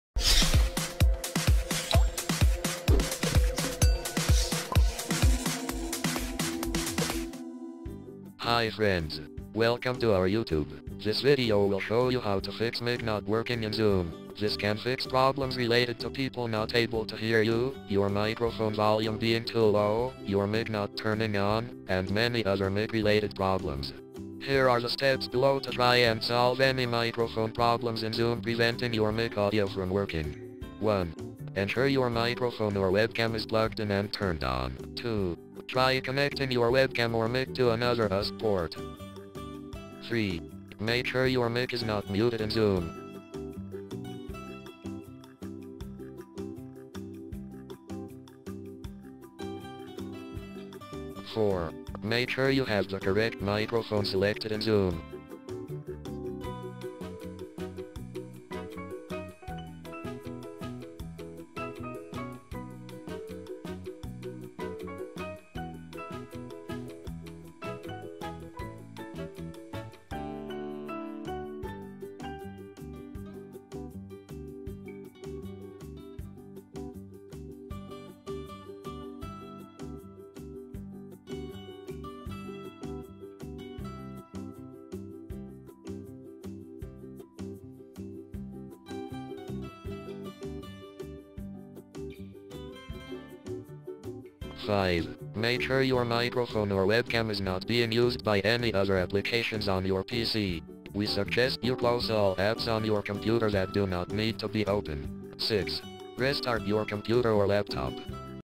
How To Fix Microphone Not Working in Zoom - Troubleshooting Microphone Issues
This video will show you how to fix mic not working in Zoom. This can fix problems related to people not able to hear you, your microphone volume being too low, your mic not turning on, and many other mic related problems.
Here are the steps below to try and solve any microphone problems in Zoom preventing your mic audio from working.
How to Fix Mic Issues in Zoom - Troubleshoot Microphone Not Working Zoom
1. Ensure your microphone or webcam is plugged in and turned on.
2. Try connecting your webcam or mic to another USB port.
3. Make sure your mic is not muted in Zoom.
4. Make sure you have the correct microphone selected in Zoom.
5. Make sure your microphone or webcam is not being used by any other applications on your PC. We suggest you close all apps on your computer that do not need to be open.
6. Restart your computer or laptop.